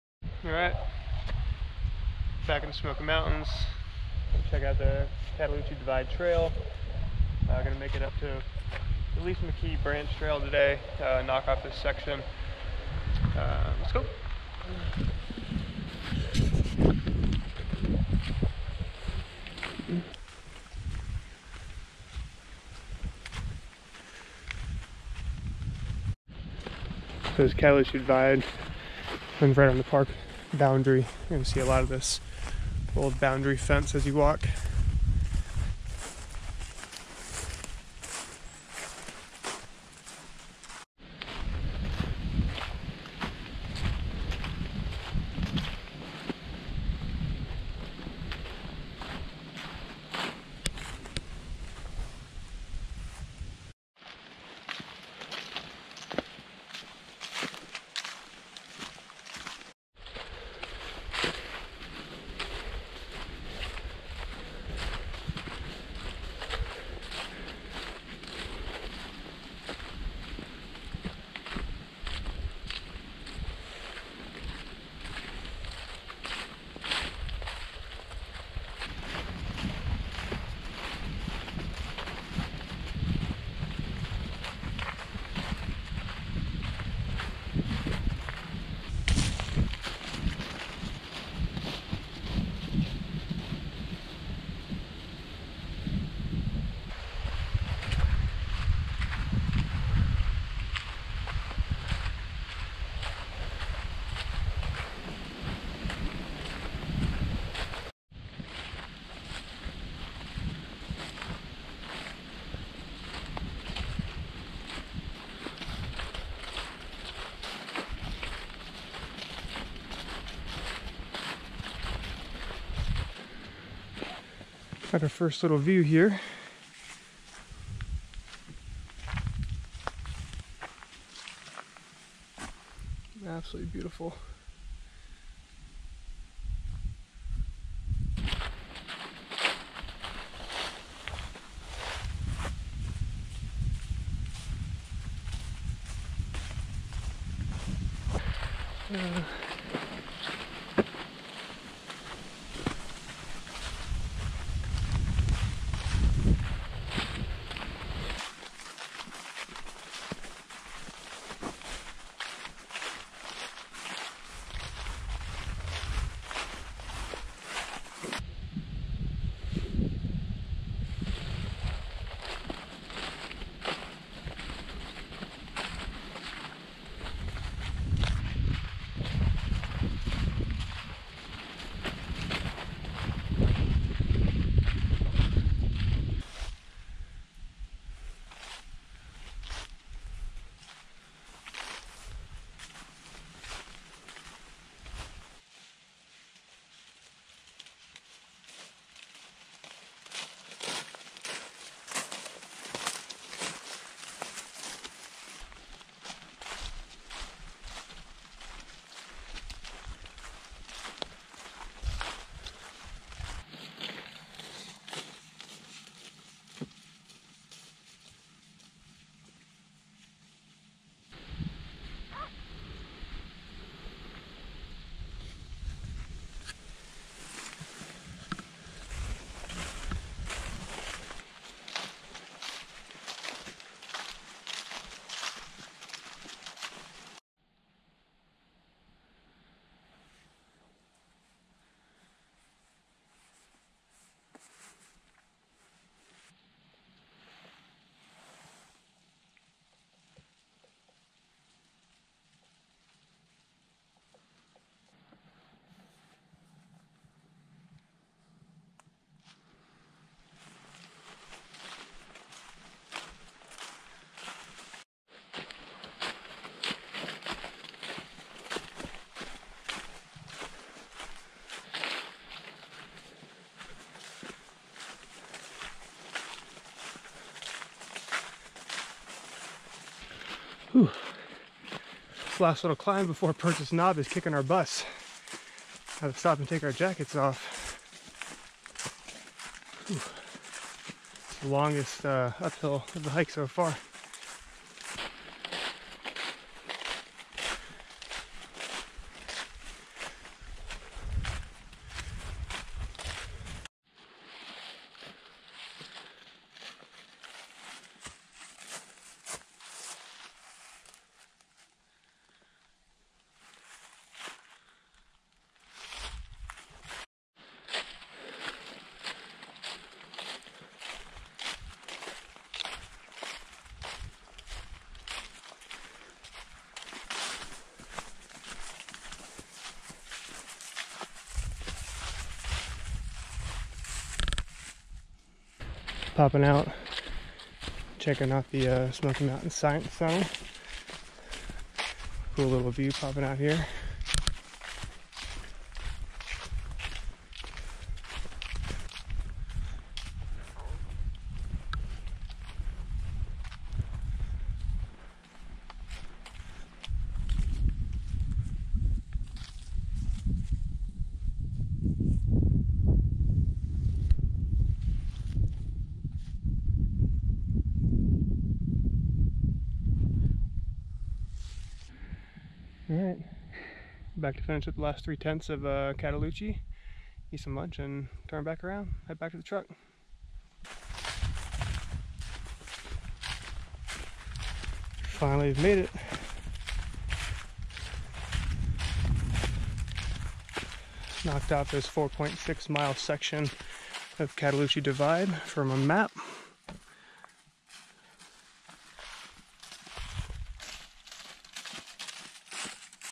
Hiking the Cataloochee Divide Trail from Cove Creek Gap to McKee Branch - Great Smoky Mountains NP
We hiked the Cataloochee Divide Trail from Cove Creek Gap to the junction with McKee Branch Trail on a beautiful November morning. The leaves were past peak in this area when we hiked this trail but there were still pockets of beautiful color as we followed the park boundary fence for a peaceful 9.2 mile round trip hike where we didn’t run into one single hiker the entire day!

As usual with my videos they aren’t anything fancy, no music and minimal narration. Just the sights and sounds of the Great Smoky Mountains.

If you plan to visit this trail, or any others, please educate yourself on the 7 Principles of Leave No Trace (LNT).